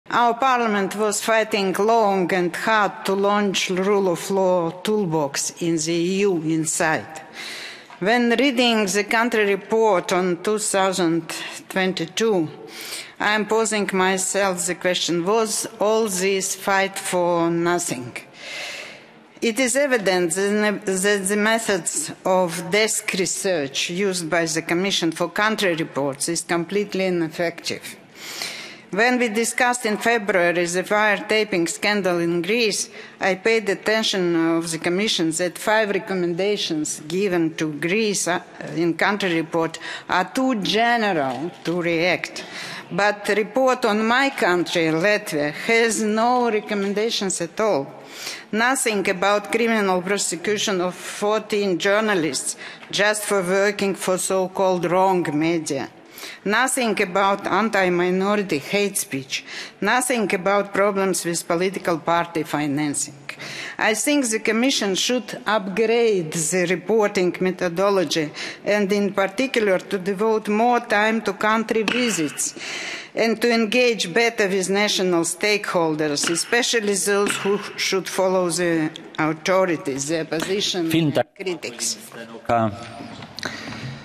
ВЕРХОВЕНСТВО ЗАКОНА ДЛЯ ВСЕХ!
"В отчете по странам для моей страны, для Латвии Еврокомиссия вообще не дает рекомендаций: ни слова об уголовном преследовании 14 журналистов лишь за их работу на, так называемые, «неправильные» СМИ, ни слова о разжигании межнациональной ненависти, ни слова о проблемах с финансированием политических партий", – с такими словами наш депутат Европарламента Татьяна ЖДАНОК обратилась к Еврокомиссии во время сессии в Брюсселе.